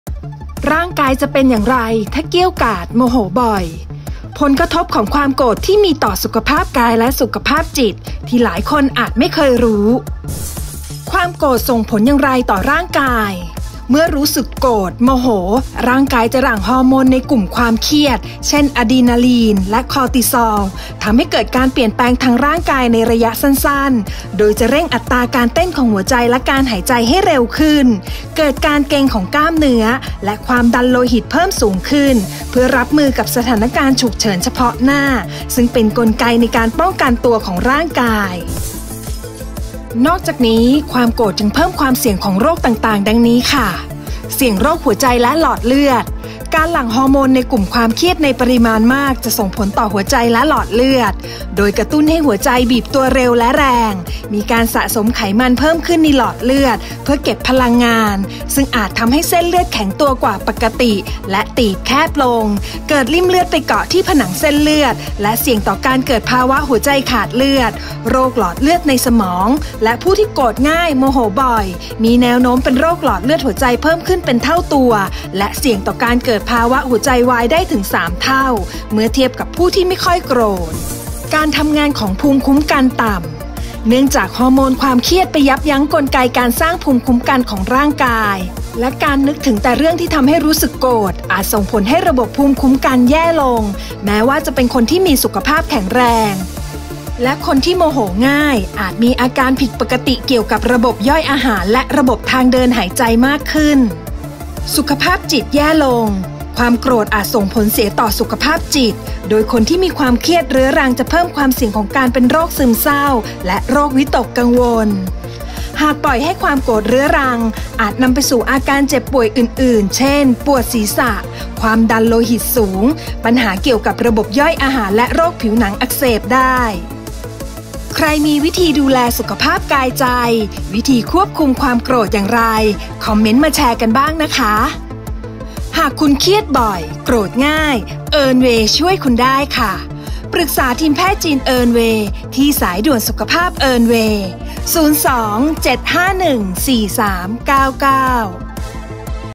เกรี้ยวกราดโมโหบ่อย ร่างกายจะเป็นอย่างไร สาระดีๆ จากแพทย์จีน Armony เอินเวย์
เกรี้ยวกราดโมโหบ่อย ควบคุมตัวเองไม่ค่อยได้ สุขภาพร่างกายจะเป็นอย่างไร และจะเสี่ยงต่อโรคอะไรมากขึ้นบ้าง คลิปนี้เอินเวย์มีคำตอบค่ะ

สนใจปรึกษาปัญหาสุขภาพหรือวิธีการดูแลสุขภาพตามทัศนะการแพทย์แผนจีนสามารถปรึกษาได้กับทีมแพทย์จีนเอินเวย์ ผ่าน Line OA เพียงคลิก